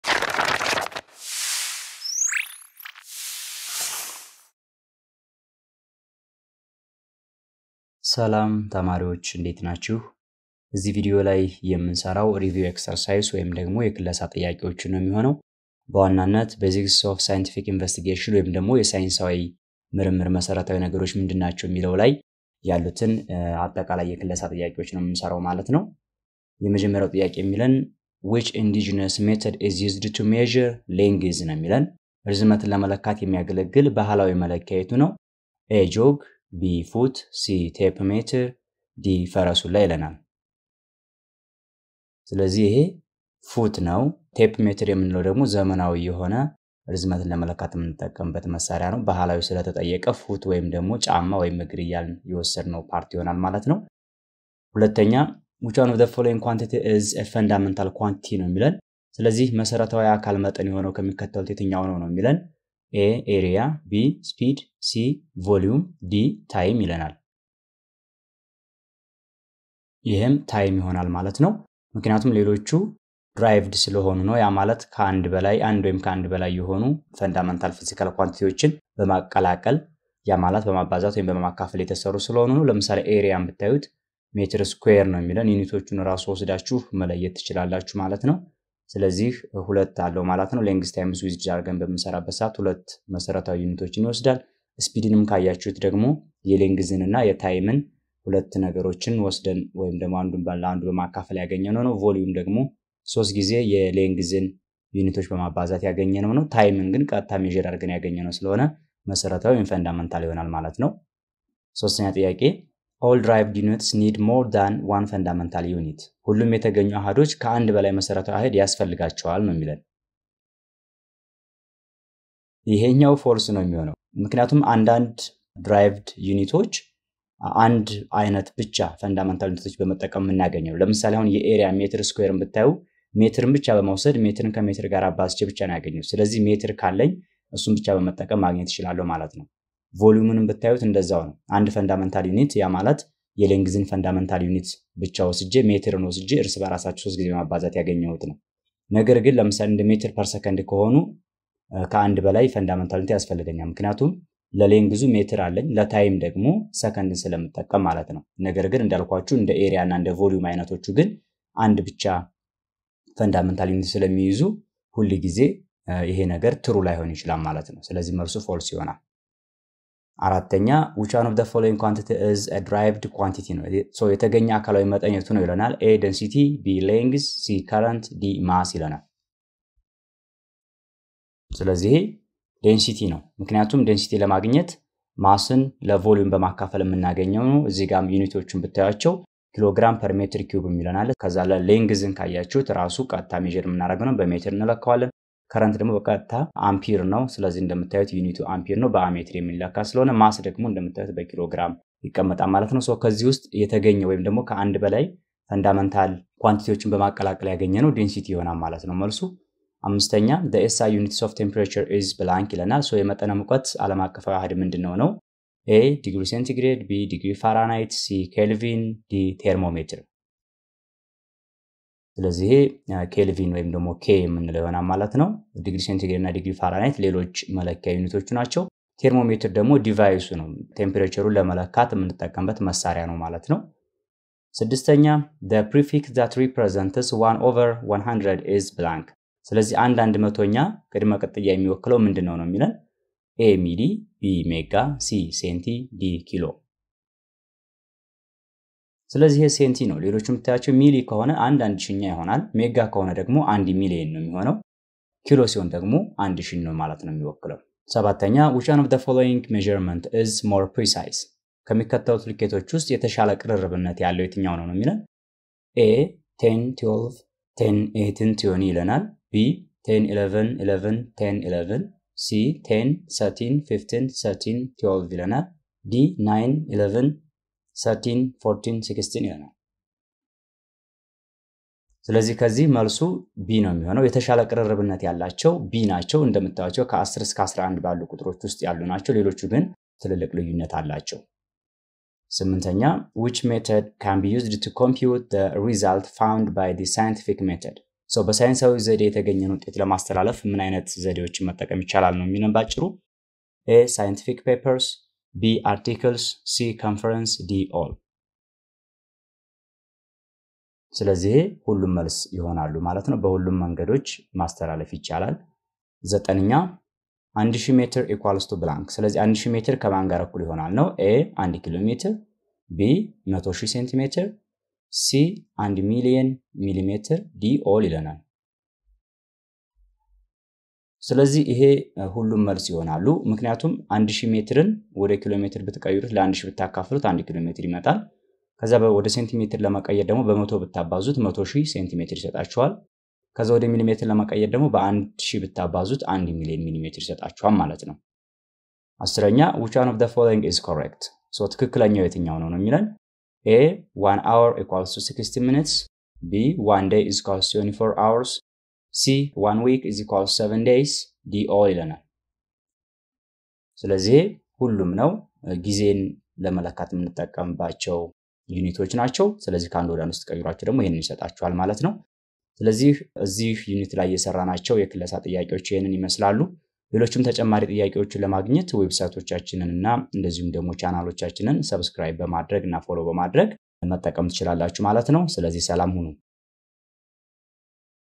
Grade 8 General Science Unit 1: Review Exercises | የክለሳ ጥያቄዎች | Saquama | ሳቋማ
New Ethiopian Curriculum of Grade 8 General Science Unit 1: Basics of Scientific Investigation ǀ Saquama ሳቋማ ǀ

This video is based on New Ethiopian Curriculum of Grade 8 General Science Unit 1: Basics of Scientific Investigation. In this video, we will do review questions on the basics of scientific investigation, covering the whole unit. በዚህ ቪዲዮ ሳይንሳዊ ምርምር ላይ ያተኮሩ እና በዚህ ዩኒት ላይ የሚያጠነጥኑ የክለሳ ጥያቄዎችን እንሰራለን።